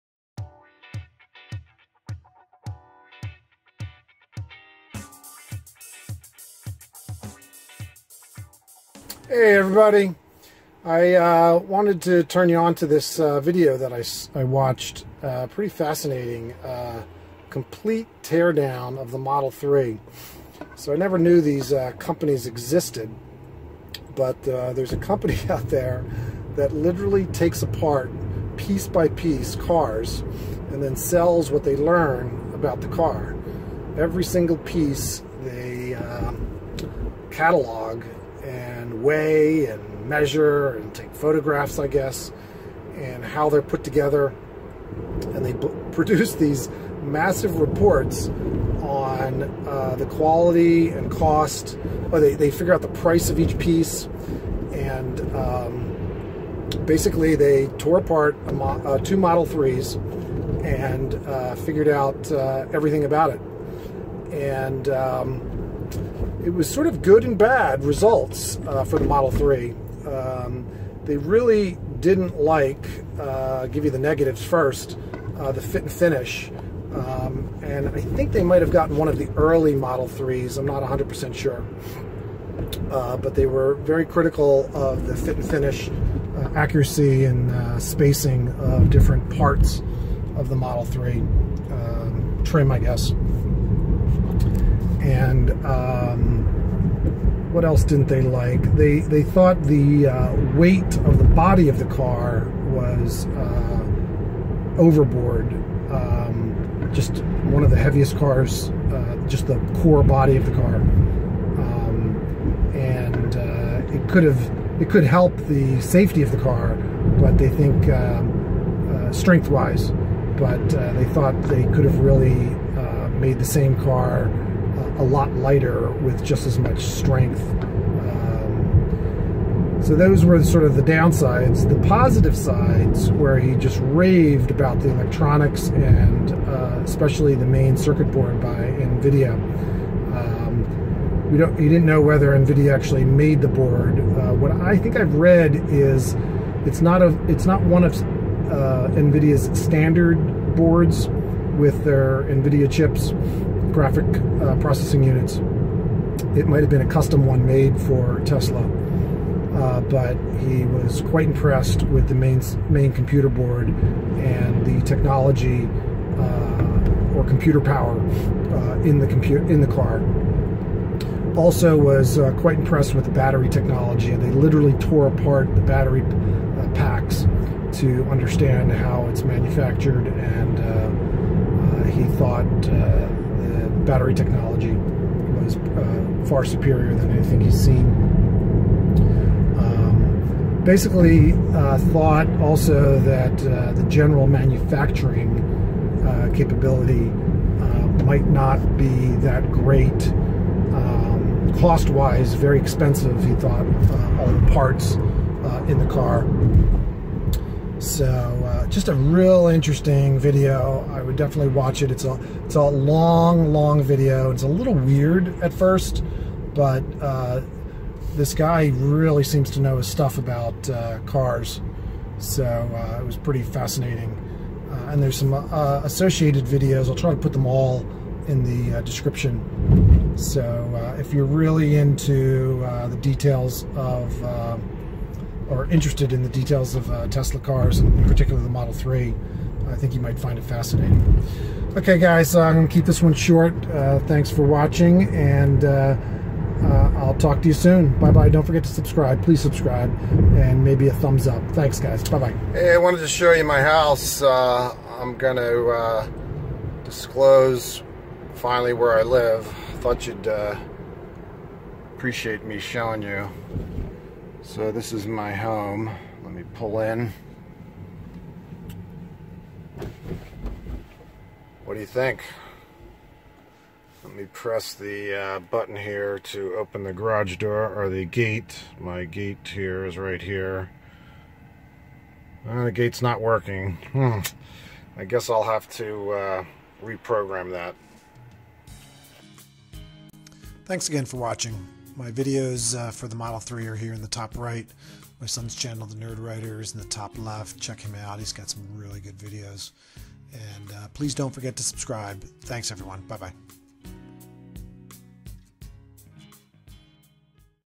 Tesla Model 3 Tear Down and Analysis - Nick's Model 3 - Day 39
Day 39 - I talk about a set of videos I watched about a complete tear down and DETAILED analysis of the Tesla Model 3. Sandy Munro from Munro & Associates discusses the Model 3, he finds a lot of bad things and good things so give the videos a chance. Sandy seems to really know the automotive business. He really nails Tesla on their fit and finish, but at the same time gives it great praise for the performance, handling, and technology so I feel it was a fair analysis.